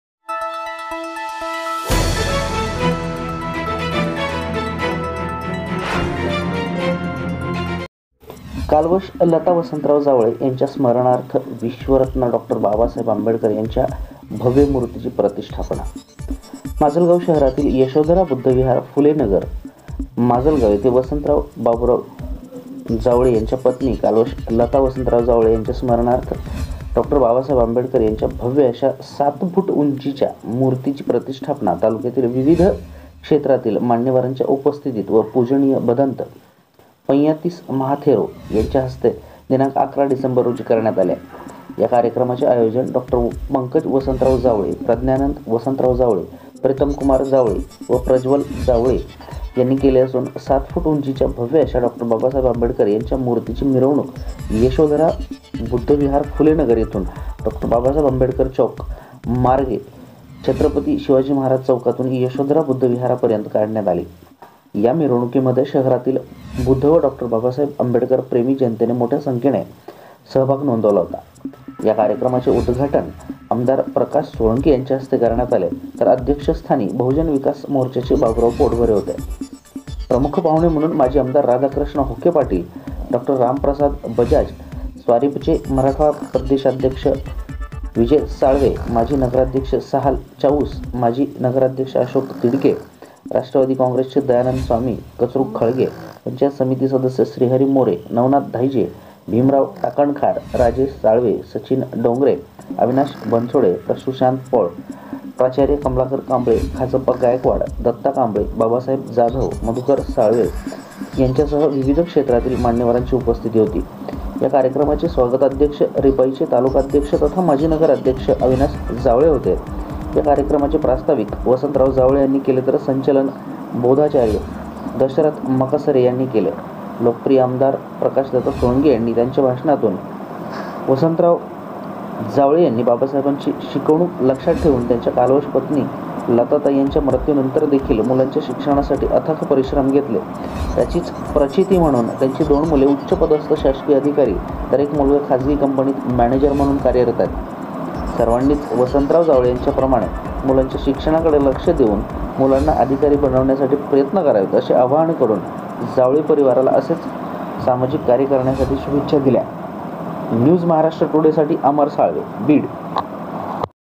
का.लता जावळे यांच्या स्मरणार्थ डॉ.बाबासाहेब आंबेडकर यांच्या भव्य मुर्तिची स्थापना
कालवश लता वसंतराव जावळे यांच्या स्मरनार्थ विश्वरत्न डॉ. बाबासाहेब आंबेडकर यांच्या भव्य मूर्तीची प्रतिष्ठापना.

माजलगांव : ( प्रतिनिधी ) शहरातील यशोधरा बुद्ध विहार फुले नगर माजलगांव येथे वसंतराव बाबुराव जावळे यांच्या पत्नी  कालवश लता वसंतराव जावळे यांच्या स्मरनार्थ विश्वरत्न डॉ. बाबासाहेब आंबेडकर यांच्या भव्य अशा o७ फुट उंचीच्या मूर्तीची प्रतिष्ठापना तालुक्यातील विविध क्षेत्रातील मान्यववरांच्या उपस्थीतीत व पूजनिय भदंत पंय्यातीस महाथेरो ( सिरसाळा ) यांच्या हस्ते दिनांक ११ डिसेंबर रोजी करण्यात आली आहे.
या कार्यक्रमाचे आयोजन डॉ. पंकज वसंतराव जावळे , प्रज्ञानंद वसंतराव जावळे , प्रितमकुमार वसंतराव जावळे व प्रज्वल वसंतराव जावळे यांनी केले असून सात फूट उंचीच्या भव्य अशा डॉ. बाबासाहेब आंबेडकर यांच्या मुर्तीची मिरवणूक यशोधरा बुद्ध विहार फुलेनगर येथून डॉ. बाबासाहेब आंबेडकर चौक मार्गे छत्रपती शिवाजी महाराज चौकातुन यशोधरा बुद्ध विहारा पर्यंत काढण्यात आली. या मिरवणूकी मध्ये शहरातील बुद्ध व डॉ.आंबेडकर प्रेमी जनतेने मोठ्या संख्येने सहभाग नोंदवला होता.
या कार्यक्रमाचे उद्घाटन आमदार प्रकाश सोळंके यांच्या हस्ते करण्यात आले तर अध्यक्ष स्थानी बहूजन विकास मोर्चाचे बाबूराव पोटभरे होते. प्रमुख पाहूने म्हणून माजी आमदार राधाकृष्ण होके पाटिल डॉ. रामप्रसाद बजाज स्वारिप चे मराठवाडा प्रदेशशाध्यक्ष विजय साळवे माजी नगराध्यक्ष सहाल चाऊस माजी नगराधक्ष अशोक तिडके राष्ट्रवादी काँग्रेसचे दयानंद स्वामी , कचरू खळगे , पं.स. सरस्य श्रीहरी मोरे , नवनाथ धाईजे , भिमराव टाकणखार , राजेश साळवे , सचिन डोंगरे . अविनाश बनसोडे , सुशांत पौळ , प्राचार्य कमलाकर कांबळे , खाजप्पा गायकवाड , दत्ता कांबळे , बाबासाहेब जाधव , मधूकर साळवे सह विविध क्षेत्रातील मान्यवर उपस्थीत होते. या कार्यक्रमाचे स्वागताध्यक्ष रिपाईचे तालुकाध्यक्ष तथा माजी नगराध्यक्ष अविनाश जावळे होते. या कार्यक्रमाचे प्रस्तावीक आयु वसंतराव जावळे यांनी केले तर संचलन बोधाचारी दशरथ मकसरे यांनी केले. लोकप्रिय आमदारा प्रकाश दादा सोळंके ह्यांनी त्यांचे भाषणातून, श्रीयुत वसंतराव बाबुराव जावळे यांनी बाबासाहेबांची शिकवणूक लक्षात ठेवुन त्यांच्या कालवश पत्नी लताताई यांच्या मृत्युनंतर देखील मुलांच्या शिक्षणासाठी अथक परिश्रम घेतले.  त्याचीच प्रचिती म्हणून त्यांची दोन मुले उच्चपदस्थ शासकीय अधिकारी तर, एक मुलगा खाजगी कंपनीत मॅनेजर म्हणून कार्यरत आहेत. सर्वांनीच वसंतराव जावळे यांच्या प्रमाणे मुलांच्या शिक्षणाकडे लक्ष देऊन मुलांना अधिकारी बनविण्यासाठी प्रयत्न करावेत असे आवाहन करून जावळे परिवाराला असेच सामाजिक कार्य करण्यासाठी शुभेच्छा दिल्या.